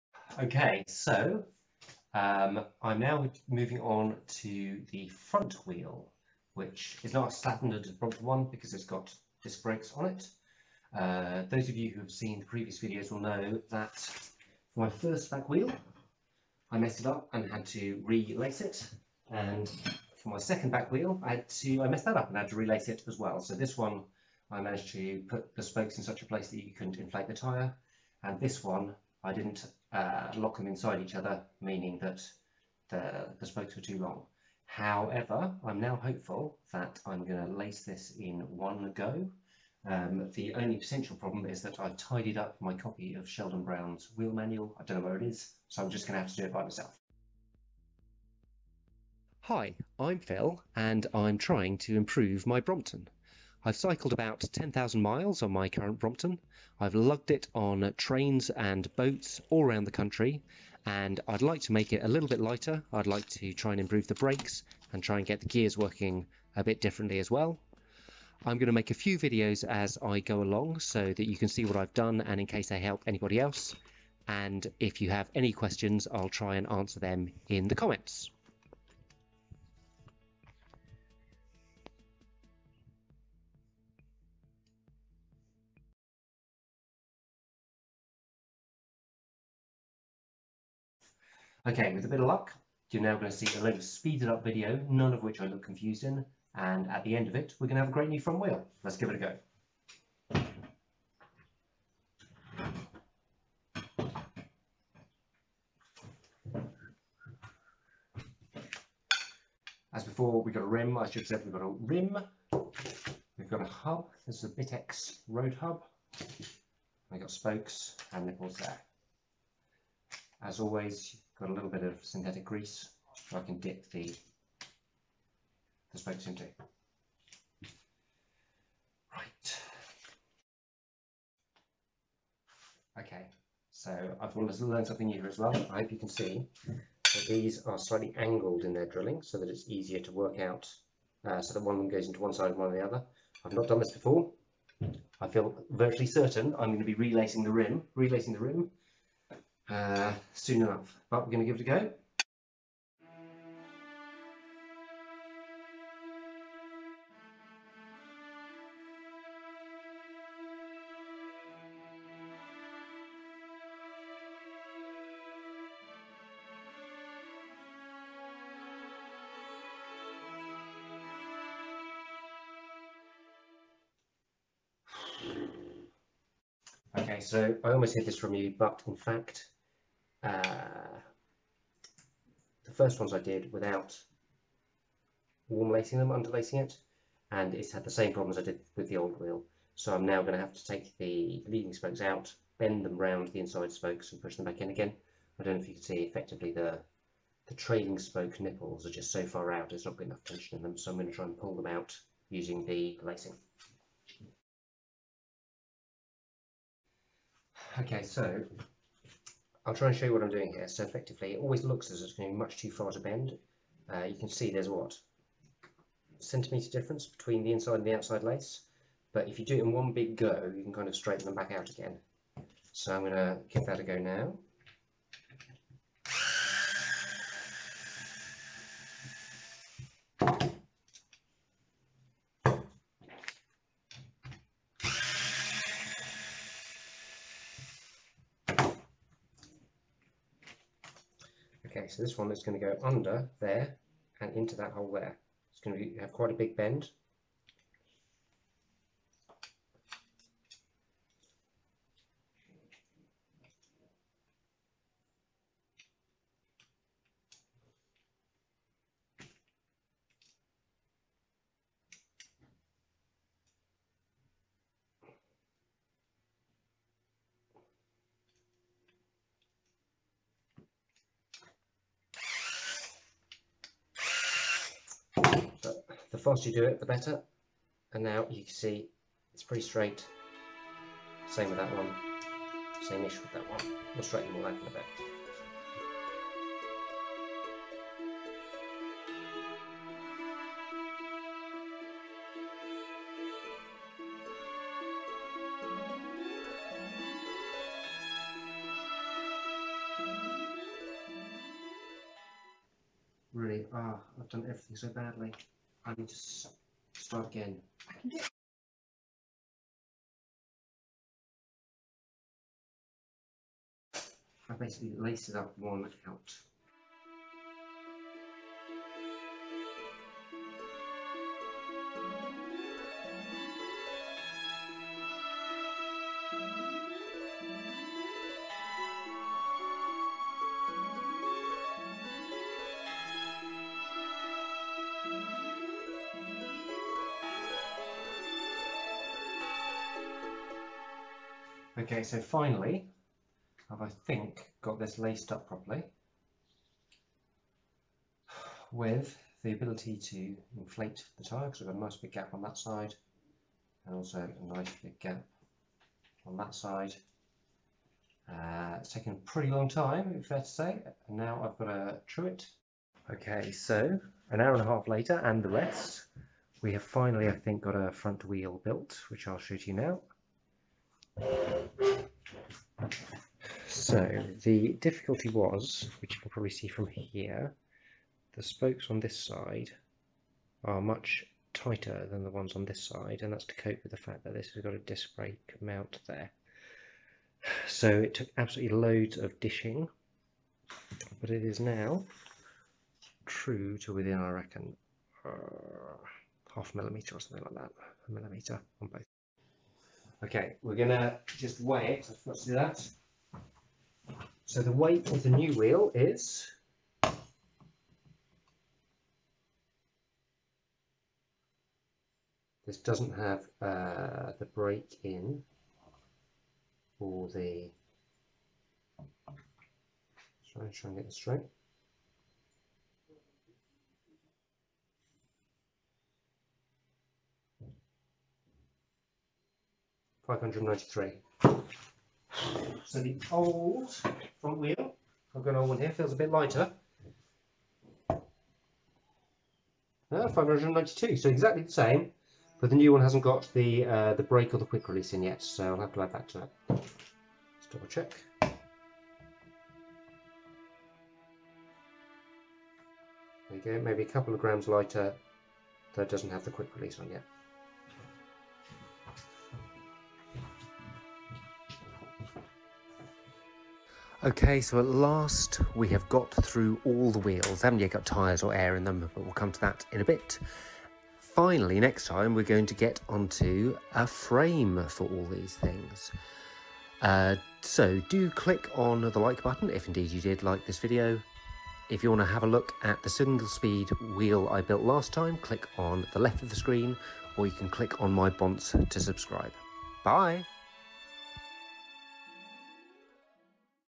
Improving (??) a Brompton stage 3 - building a disc brake front weel
Hi all - I'm trying to improve my Brompton - make it lighter, improve the brakes, and improve the gears.

This is the third in who knows how many videos - I need to build my own disc brake front wheel. Surely I won't repeat the same mistakes I made in the other two wheels?

Includes weight of the new wheel compared to the one it is replacing!

Edd spoke calculator (SJS Cycles were also very helpful)

Some camerawork: D and J Buckley.

WHEEL COMPONENTS:
Bitex BX106F hub (this hub is *super cool*)

Jtek 14g Black Plain Gauge Spokes & 12 mm Black Nipples 158mm